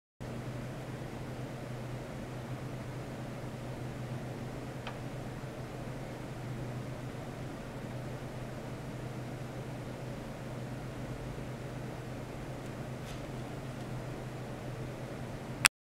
超音波の基礎実験（超音波システム研究所）
超音波の基礎実験
－超音波の伝搬による表面弾性波の検討－

伝搬する超音波の伝搬状態を

参考
超音波による音響特性テスト